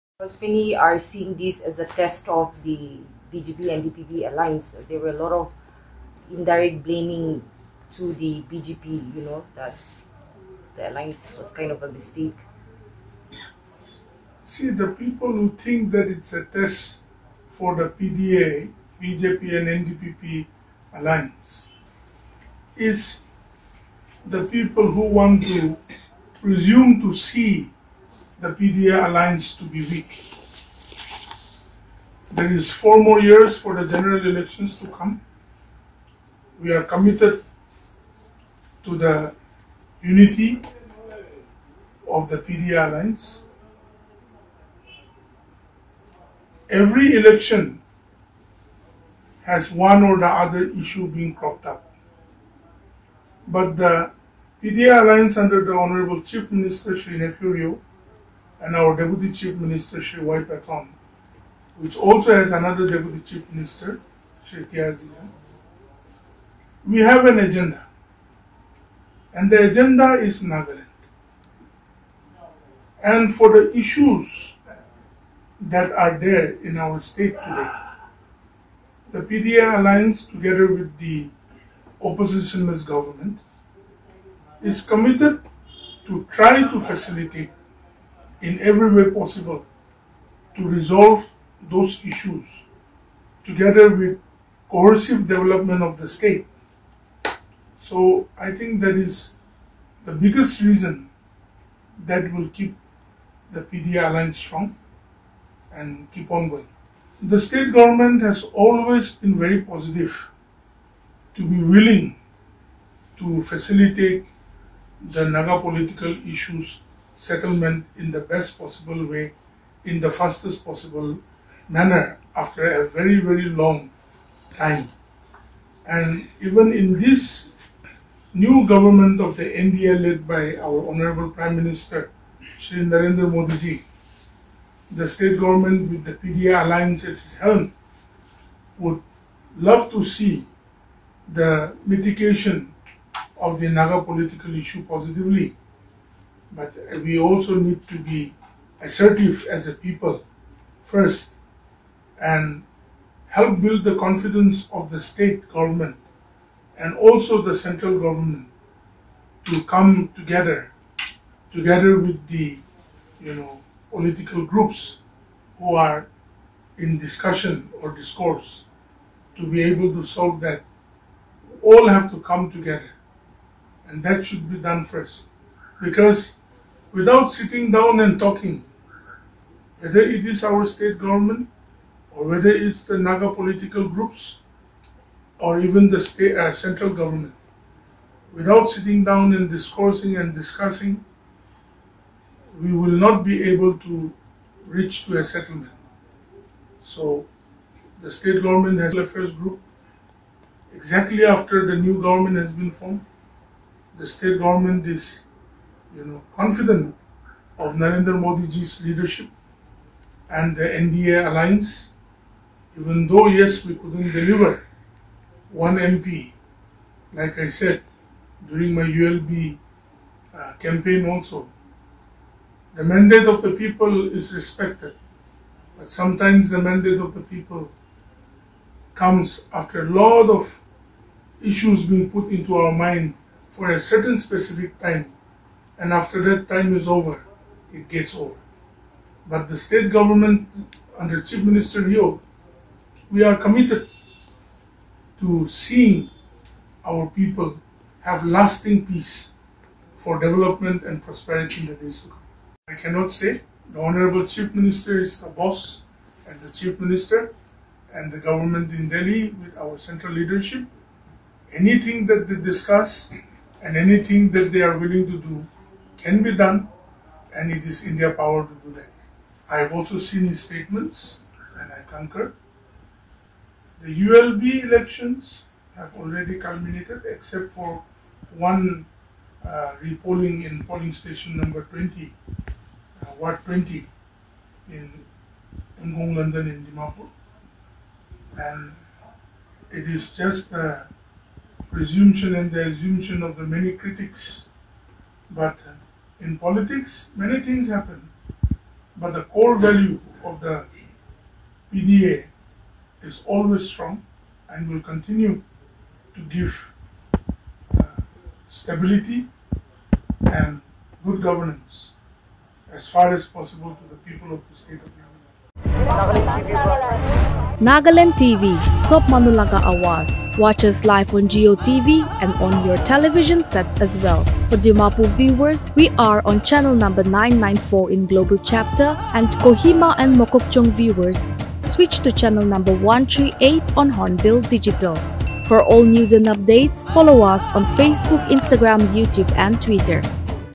IMNA ALONG ON POLITICAL RESHUFFLE AND NAGA POLITICAL ISSUE
Addressing the media on reshuffling of seats,Imna said that it's up to the Chief Minister and the government in Delhi with their central leadership. "So anything they discuss and anything they are willing to do can be done as it's in their power to them" he said. Stating on the Naga political issues he said that the state government has always in very positive to be willing to facilitate the Naga political issues settlement in the best possible way after a very long time and even in this new NDA government under the leadership of PM Modi, the state government with PDA alliance would  love to see the medication to resolved positively. He added that it also needs to be assertive as the people first and help with the confidence of the state government and also the central government to come together with the political groups who are in discussion. Asking about the test of  this ULB election between NDPP & BJP Alliance ,he said that there are four years until the general election to come and they are committed to the Unity of the PDA alliance.